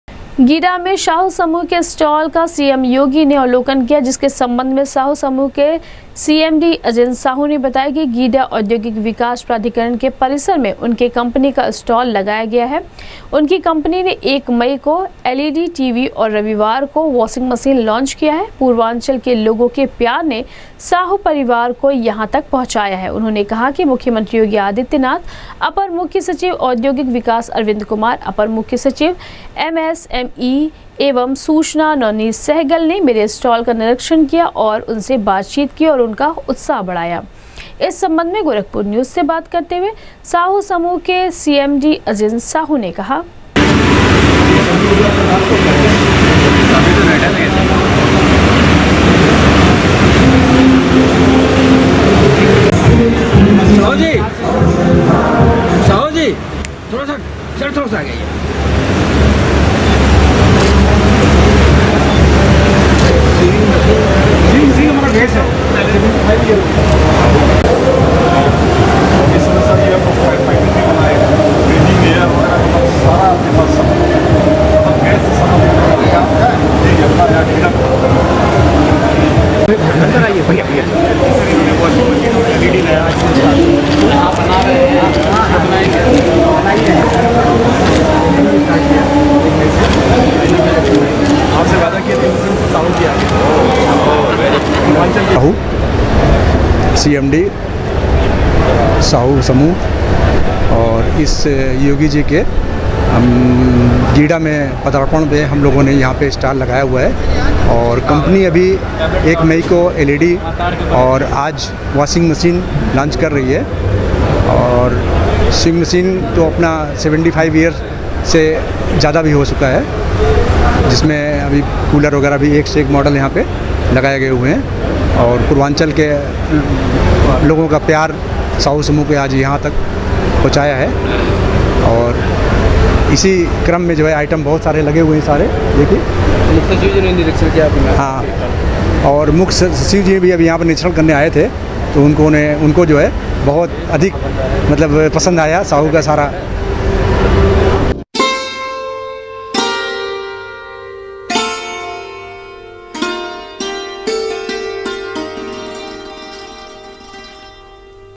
गीडा में साहू समूह के स्टाल का सीएम योगी ने किया अवलोकन,साहू समूह के सीएमडी अजिन साहू ने कहा
गीडा में साहू समूह के स्टाल का सीएम योगी ने किया अवलोकन,साहू समूह के सीएमडी अजिन साहू ने कहा, 1 मई को एलईडी टीवी और रविवार को वॉशिंग मशीन किया है लॉन्च उनकी कम्पनी ने,पूर्वांचल के लोगों से मिला है भरपूर प्यार ।।